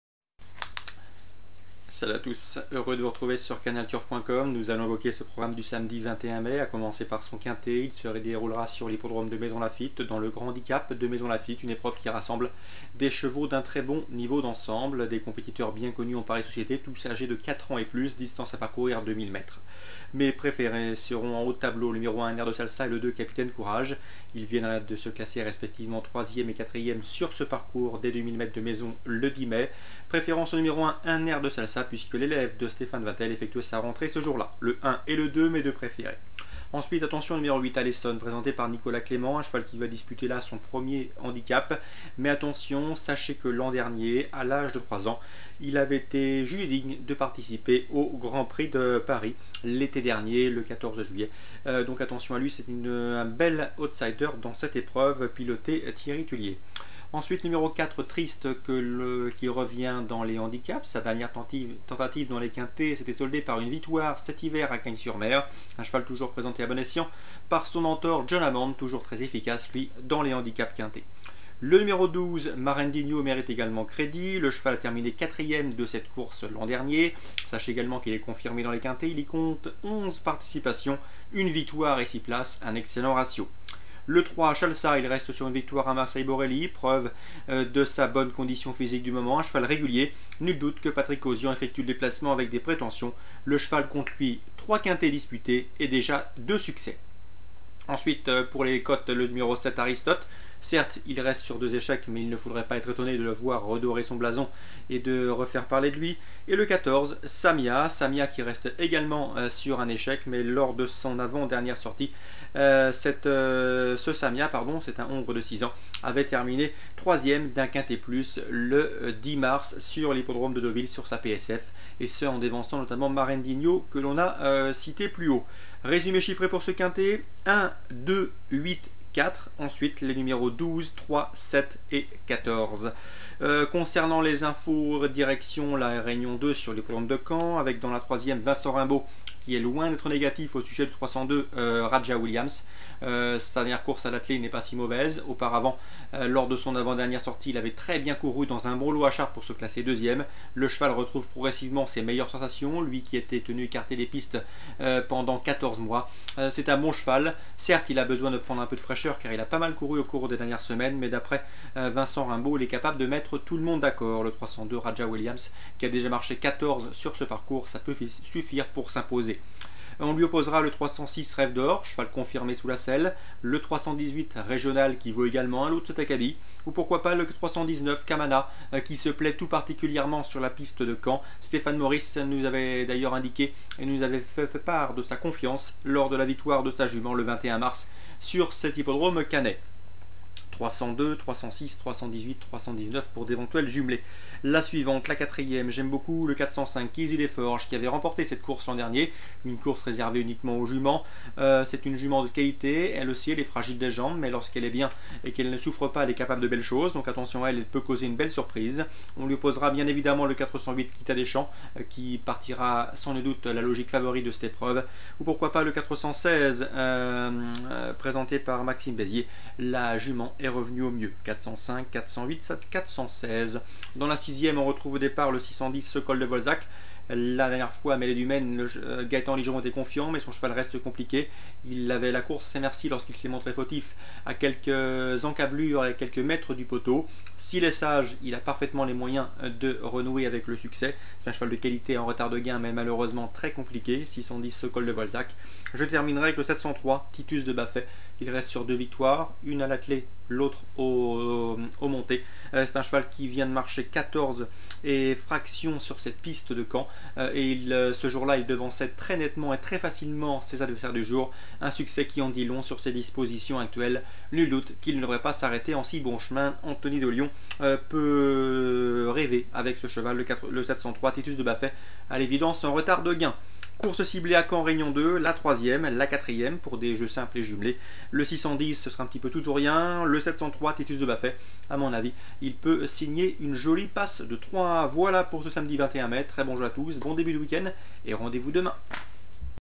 Canalturf TV - Samedi 21 Mai 2011 - GRAND HANDICAP DE MAISONS-LAFFITTE - Quinté+ PMU
Réunion 1 Course 4 - PLAT - 2000m - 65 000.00€
Quinté difficile dans un samedi difficile au total à Maisons-Laffitte. Je n'aime guère le parcours injustement prisé de 2000 mètres "ligne droite", mais force est de faire avec puisque le Grand Handicap de Maisons-Laffitte se dispute sur ce parcours déplaisant.